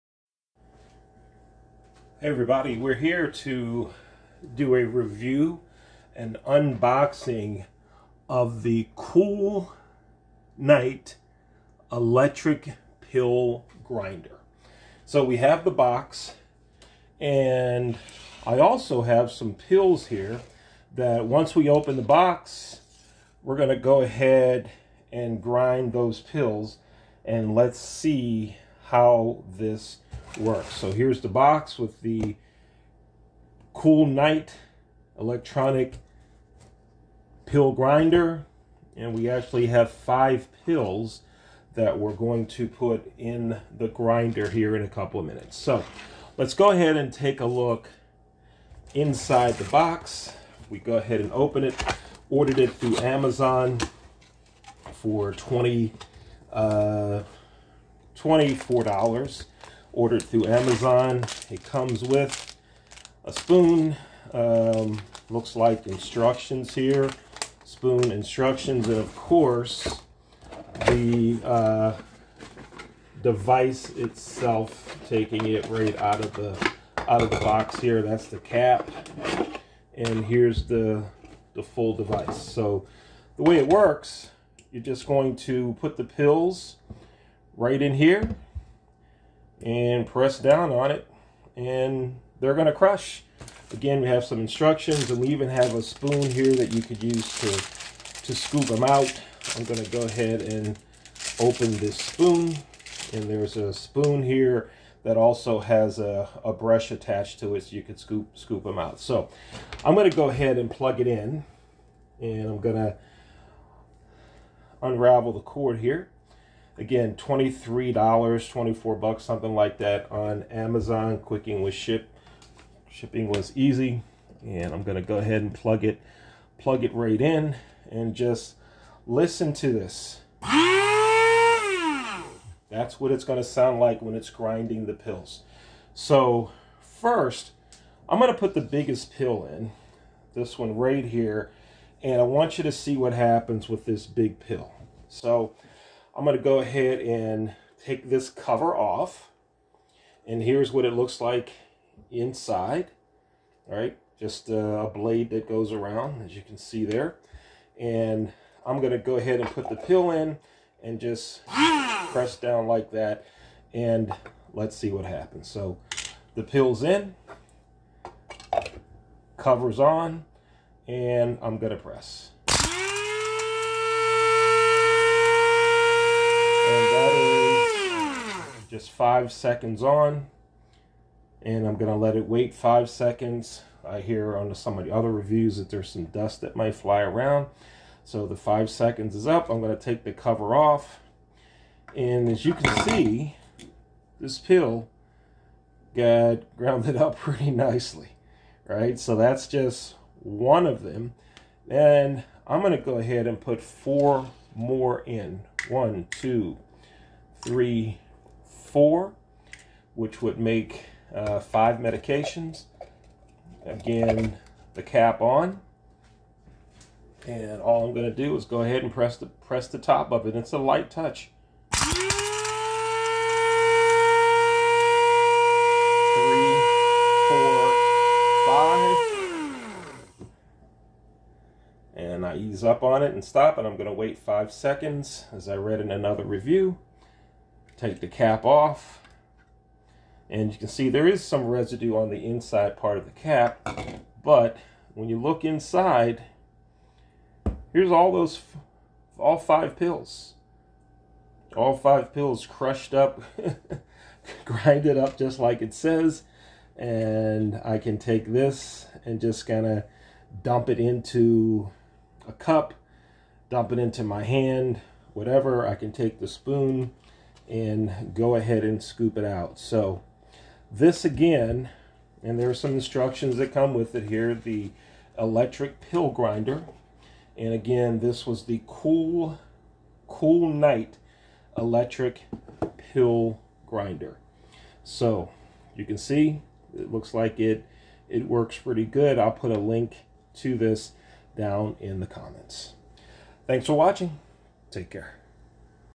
Cool Knight Electric Pill Grinder Unbox and Review
The Cool Knight Electric Pill Grinder works well!  I highly recommend it.

My elderly relatives LOVE it.

Product link